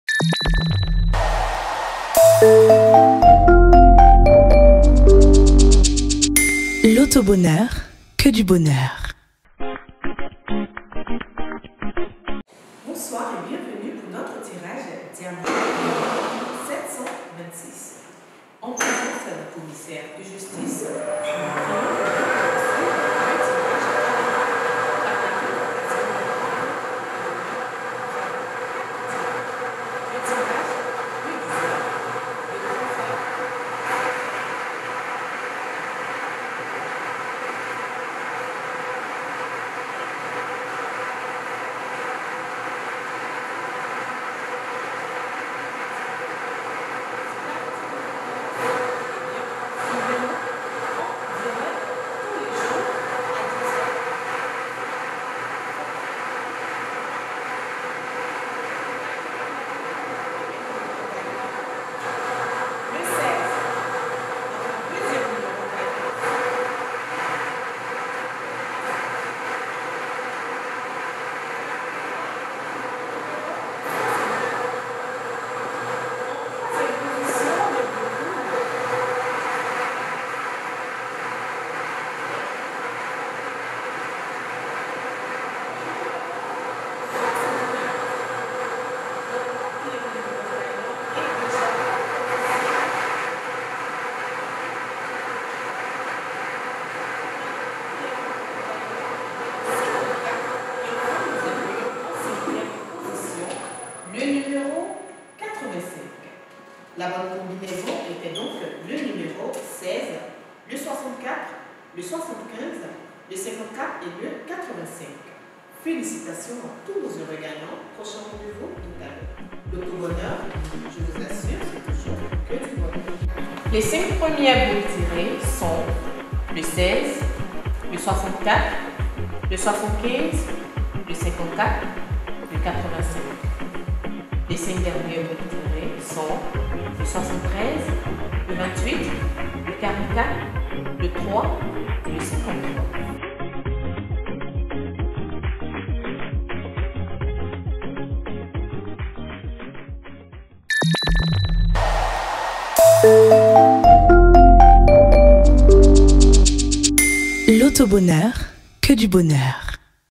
La famille Loto Bonheur, recevez la vidéo du tirage Diamant de 13 h n° 726 de ce Samedi 28/05/22.
Pour suivre le tirage en direct. Rendez-vous à la salle de tirage Lonaci aux 2 Plateaux vallons non loin de l'ambassade du Ghana ou appelez au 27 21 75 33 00.